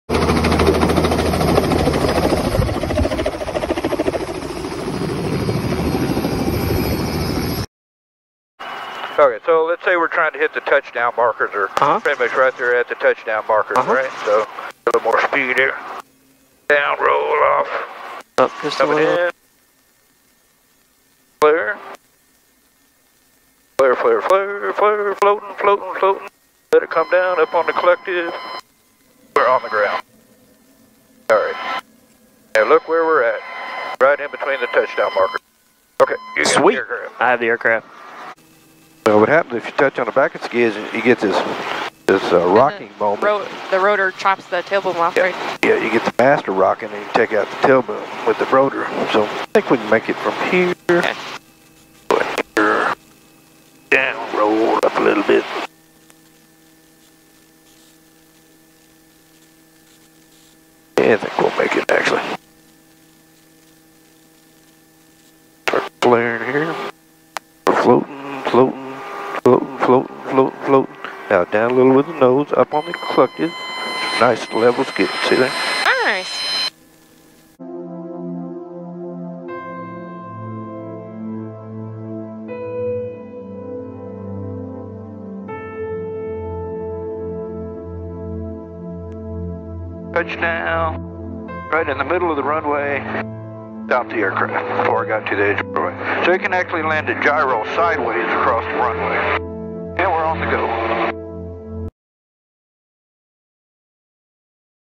Precise Landings in Helicopters and Gyroplanes
Some precise landings in Helicopters and a Gyroplane